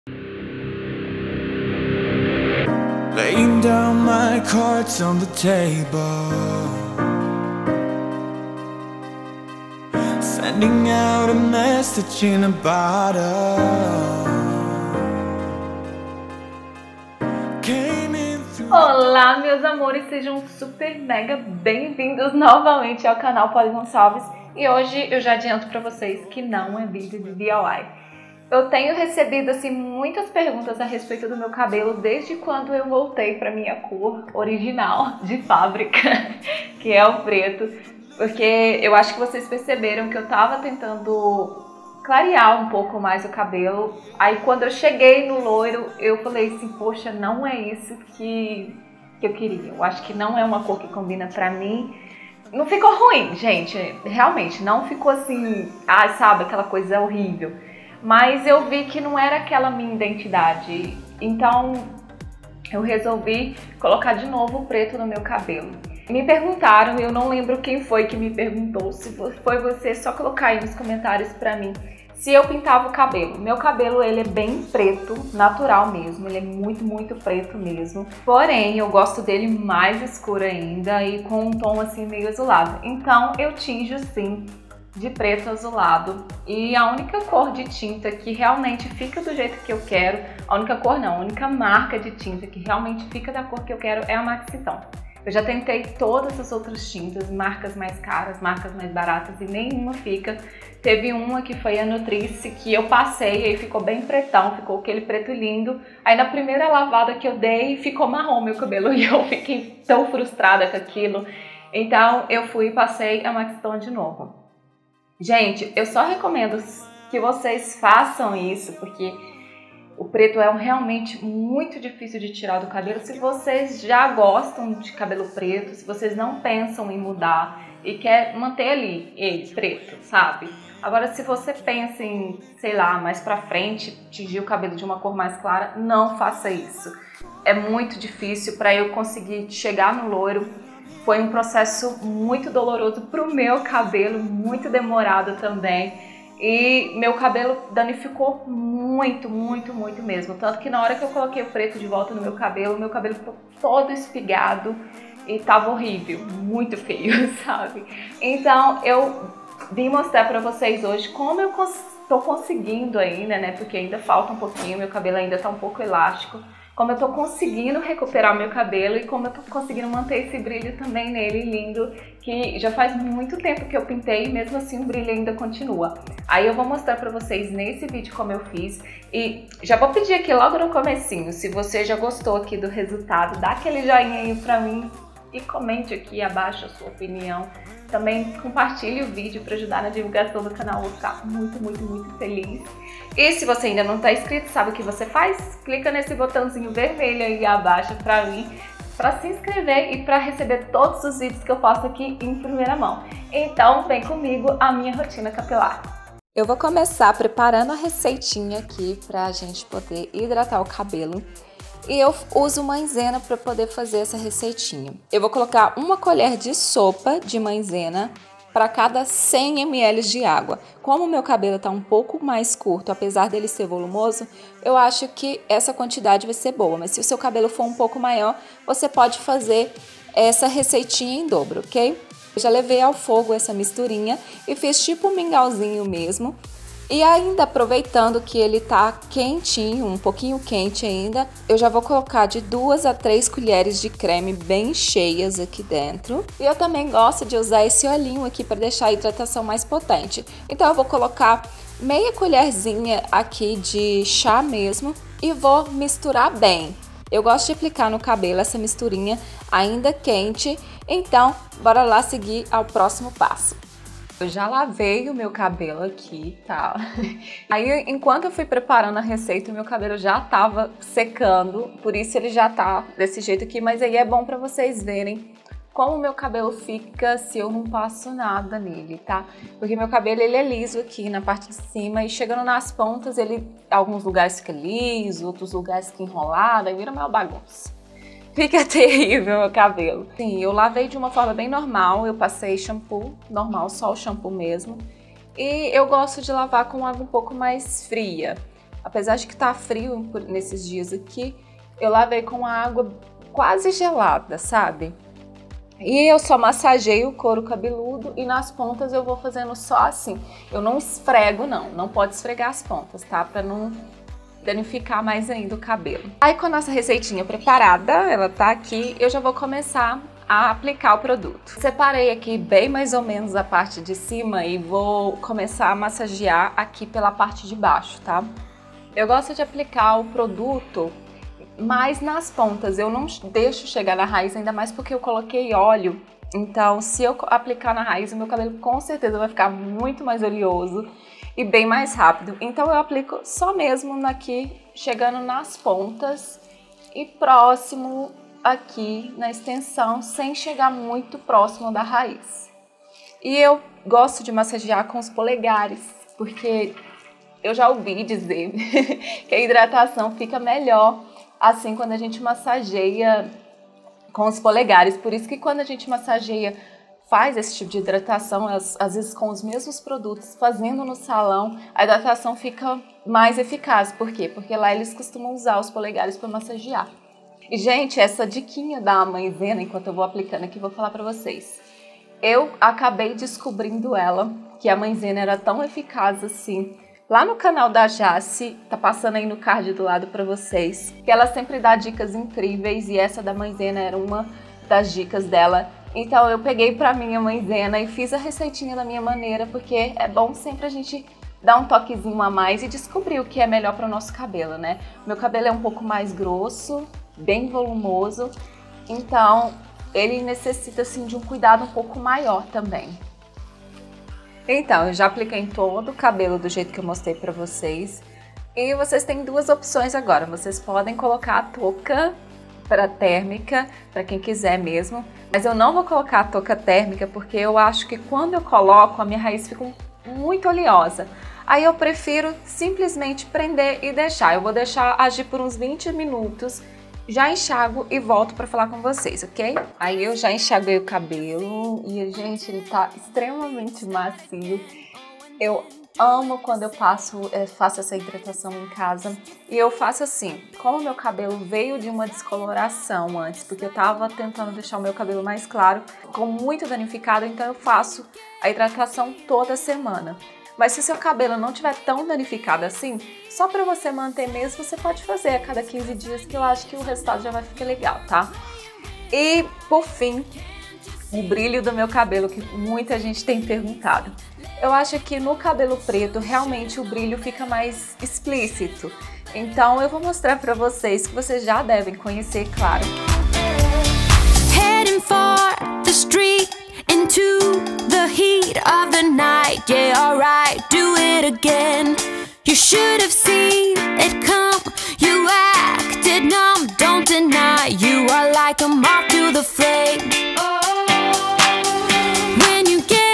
HIDRATAÇÃO COM MAISENA PODEROSA - MINHA ROTINA DE CUIDADOS COM CABELO PARTE 1 | POLY GONÇALVES
*** POR FAVOR LEIAM AQUI IMPORTANTE ***

Filmagem POLY GONÇALVES

Como estão?
Tô eu aqui hoje, toda vaidosa, pelos comentários fofos que tenho recebido ultimamente sobre meu cabelo.
Como eu disse no vídeo, eu estava em um processo de descoloração e quando consegui, não gostei do resultado final e voltei para minha cor que sempre usei que é o preto azulado.
Mas ai o estrago já estava feito, meu cabelo ficou extremamente danificado e foi me oferecido apenas uma alternativa, "cortar".
Meu cabelo já estava no tamanho que eu queria e eu não estava disposta a cortar mais, foi ai então que resolvi eu mesma começar a recuperar o meu cabelo.
E em apenas com um mês e meio de tratamento já vejo um resultado surpreendente, então nessa primeira parte do vídeo vou mostrar para vocês como faço uma hidratação realmente maravilhosa e ainda vou falara um pouco mais sobre meu cabelos e revelar alguns segredinhos.
Se gostaram compartilhem.
Beijinhos e até o próximo vídeo.

♥ HIDRATAÇÃO COM MAISENA PODEROSA - MINHA ROTINA  DE CUIDADOS COM CABELO PARTE 1 -

Receita hidratação caseira de maisena:
1 colher de sopa de Maisena - amido de milho
2 Colheres de sopa de Mascara hidrante de sua preferencia
1 Colher de sobremesa de oleo dr Marrocos Yenzah

♥ Por Poly Gonçalves ♥